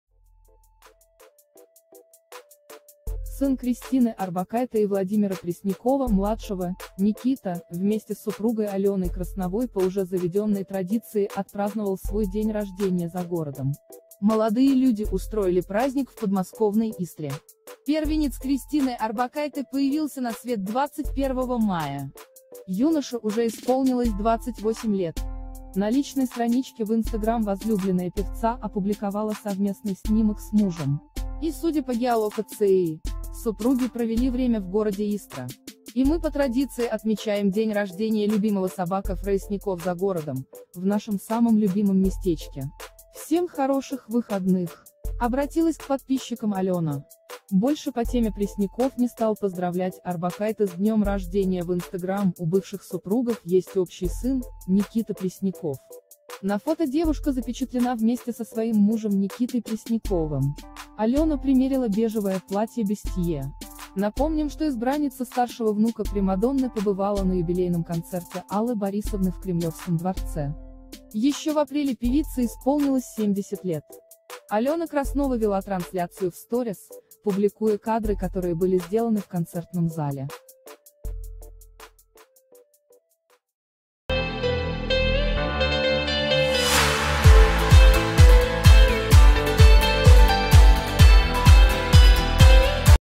Никита Пресняков с женой отметил 28-летие в подмосковной Истре
Сын Кристины Орбакайте и Владимира Преснякова-младшего — Никита — вместе с супругой Аленой Красновой по уже заведенной традиции отпраздновал свой день рождения за городом. Молодые люди устроили праздник в подмосковной Истре. Первенец Кристины Орбакайте появился на свет 21 мая. Юноше уже исполнилось 28 лет. На личной страничке в Instagram возлюбленная певца опубликовала совместный снимок с мужем. И, судя по геолокации, супруги провели время в городе Истра.

«И мы по традиции отмечаем День рождения любимого @npresnyakov за городом, в нашем самом любимом местечке.

больше по теме Пресняков не стал поздравлять Орбакайте с днем рождения в Instagram У бывших супругов есть общий сын — Никита Пресняков.

На фото девушка запечатлена вместе со своим мужем Никитой Пресняковым. Алена примерила бежевое платье-бюстье.

Напомним, что избранница старшего внука Примадонны побывала на юбилейном концерте Аллы Борисовны в Кремлевском дворце. Еще в апреле певице исполнилось 70 лет. Алена Краснова вела трансляцию в Stories, публикуя кадры, которые были сделаны в концертном зале.